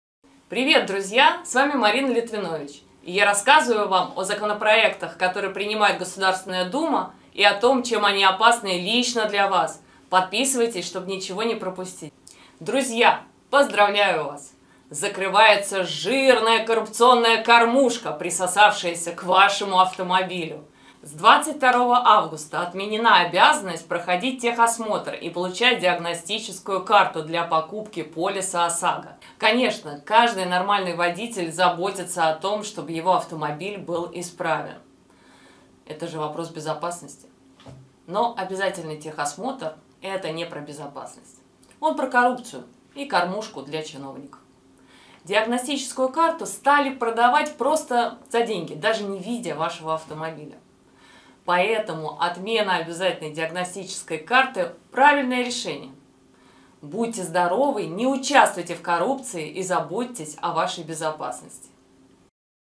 Техосмотр больше не нужен для покупки полиса ОСАГО
С 22 августа будет отменена обязанность проходить техосмотр и получать диагностическую карту для покупки полиса ОСАГО. Редкий пример разумного решения!
Полезные для автовладельцев законы тоже принимают.

ГИБДД
ПДД
ОСАГО
Полис ОСАГО
Пройти техосмотр